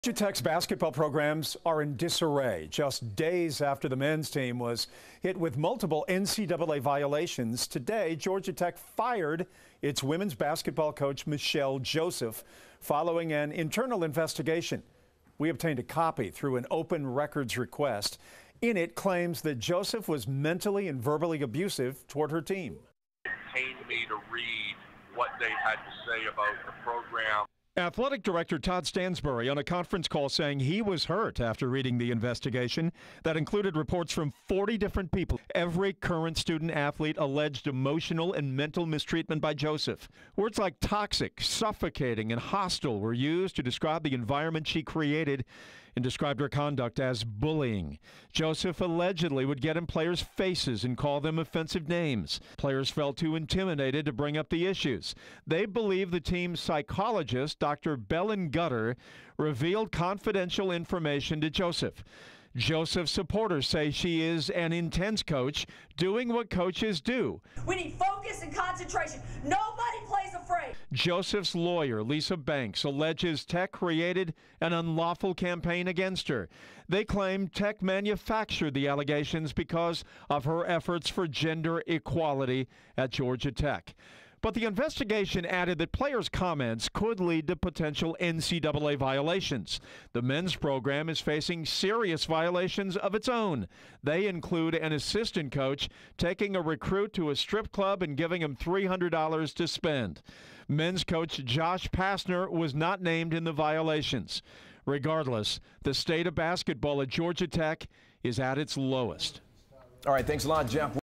Georgia Tech coach MaChelle Joseph fired
The school's basketball teams have come under renewed scrutiny after multiple recent scandals.